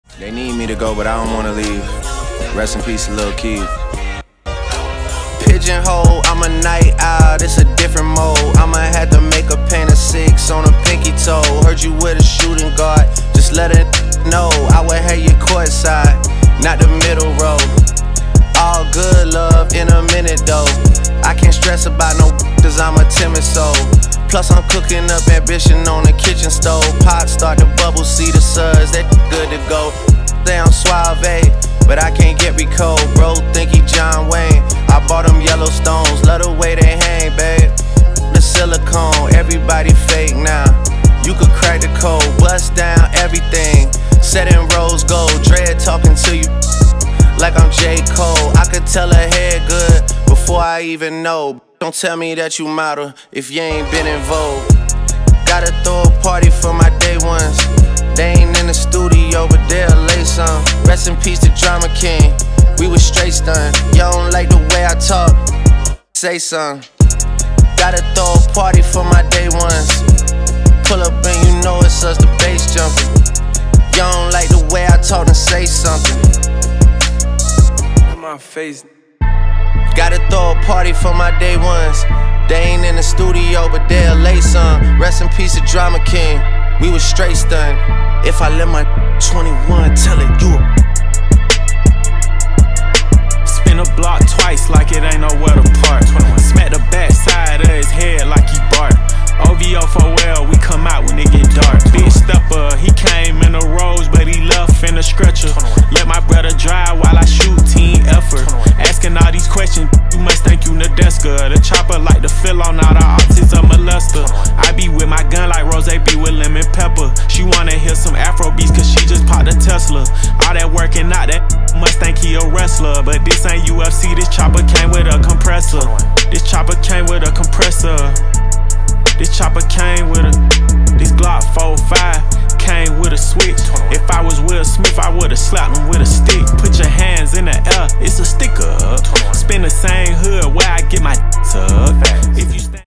Aden Chase_Indy and Canada Tourneys
The Squad had a tune up tournament in Indy before we crossed the boarder and went into Canada to show them how CE get down. The BTA tour is winding down with one last stop; enjoy these highlights of Aden doing what he does!